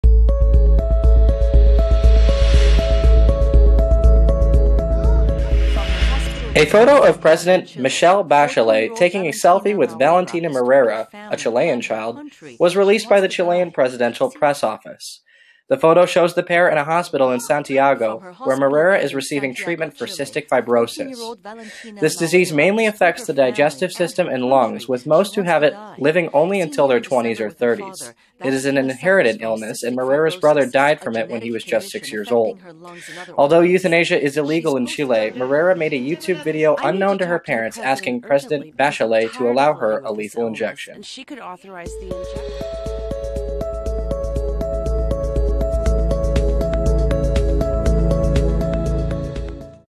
World Moved by Chilean 14-Year-Old's Plea to Be Allowed to Die
A photo of President Michele Bachelet taking a selfie with Valentina Maureira, a Chilean child, was released by the Chilean presidential press office. The photo shows the pair in a hospital in Santiago where Maureira is receiving treatment for cystic fibrosis. This disease mainly affects the digestive system and lungs with most who have it living only into their 20s or 30s. It is an inherited illness and Maureira's brother died from it when he was six-years-old. Although euthanasia is illegal in Chile, Maureira made a YouTube video unknown to her parents asking President Bachelet to allow her a lethal injection.